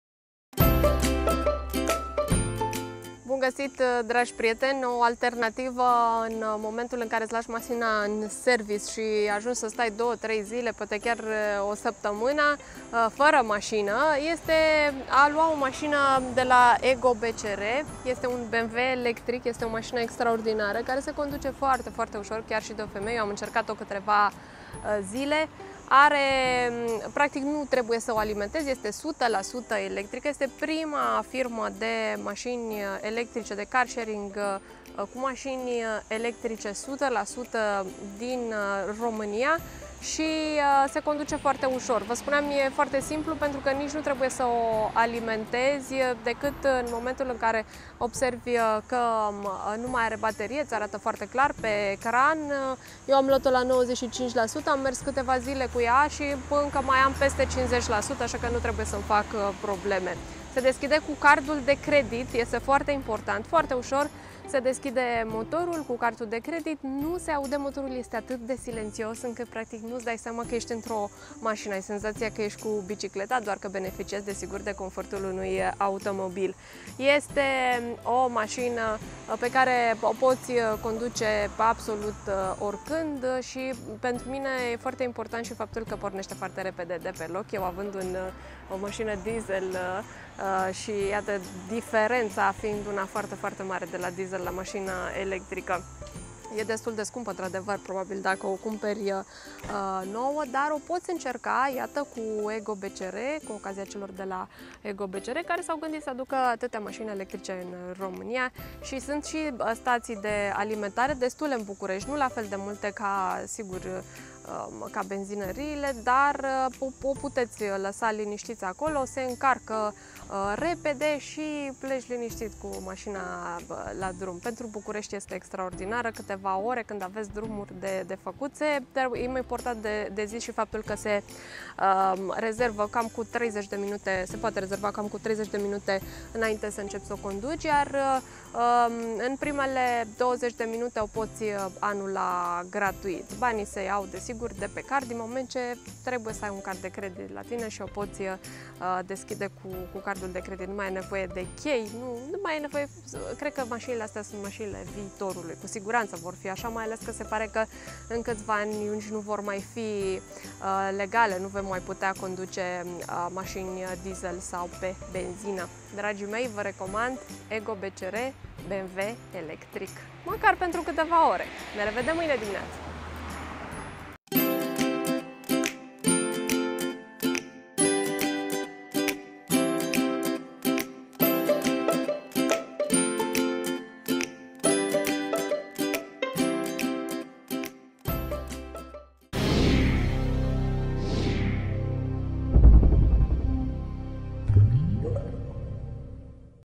Car Sharing Electric
O experianta de interesanta Car Sharing Electric cu BCR eGO